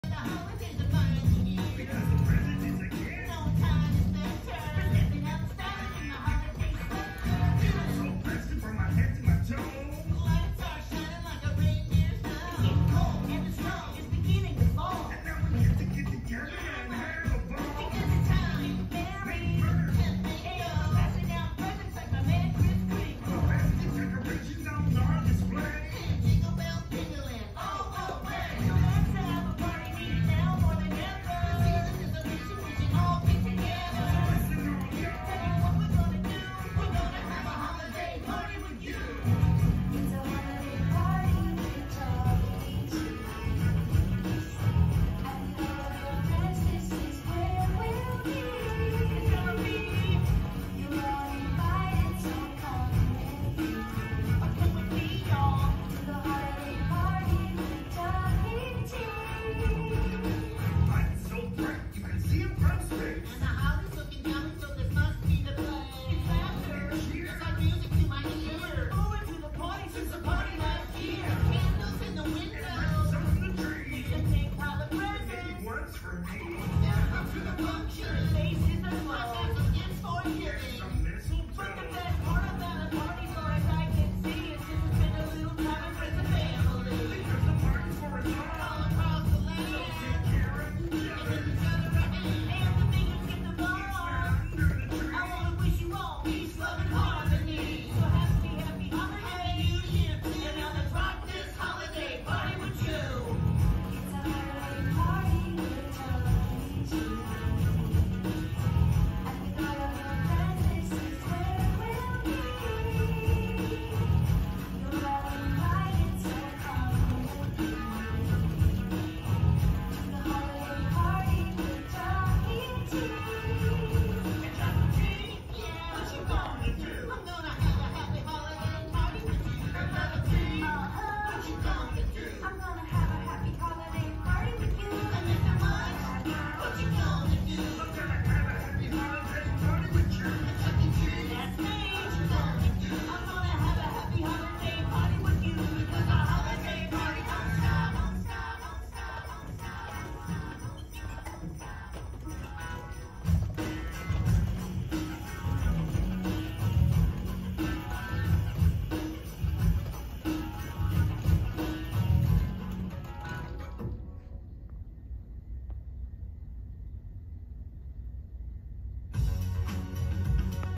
Chuck E. Cheese’s “holiday party” Middletown NJ
Stage looks great! Sorry the beginning got cut off
Filmed 11/21/2022